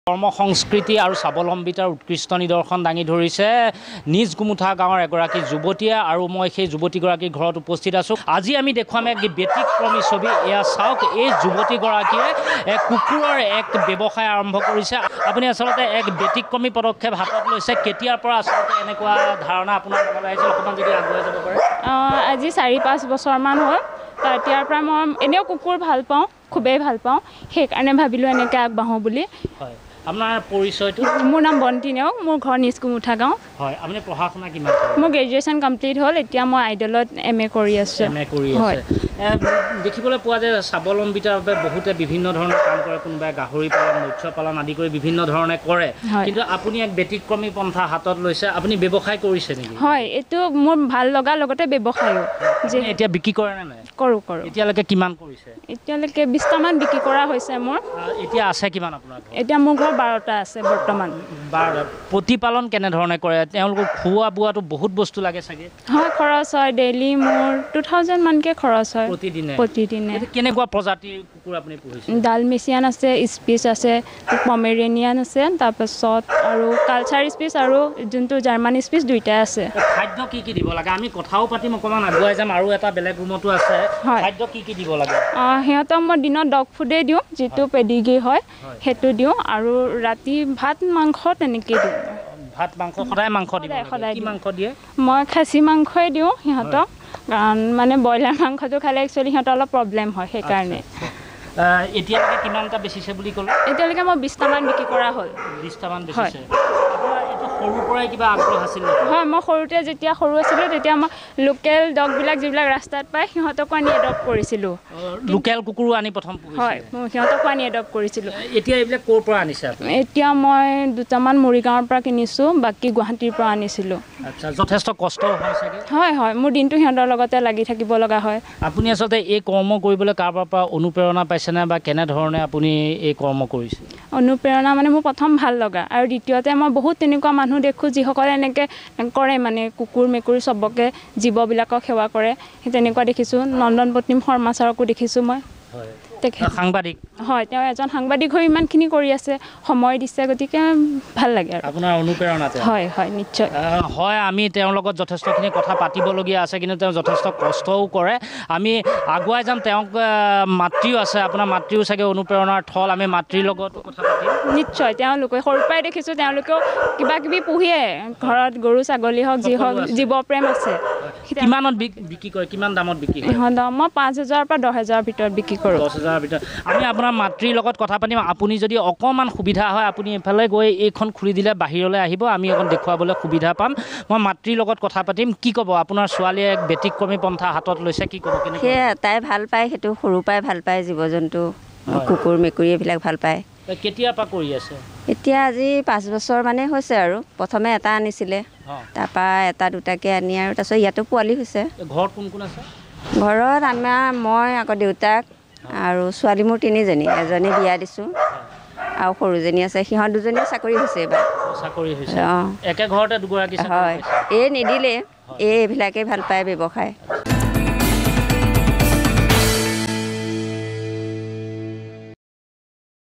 কুকুৰ পালন কৰি স্বাৱলম্বীতাৰ দিশত আগুৱাইছে নগাঁও জিলাৰ বঢ়মপুৰ-গোমোঠাগাঁৱৰ বন্তি নেওগ
ব্যৱসায়িক ভিত্তিত কুকুৰ পালন কৰি স্বাৱলম্বীতাৰ দিশত আগুৱাইছে নগাঁও জিলাৰ বঢ়মপুৰ-গোমোঠাগাঁৱৰ উচ্চশিক্ষিতা যুৱতী বন্তি নেওগ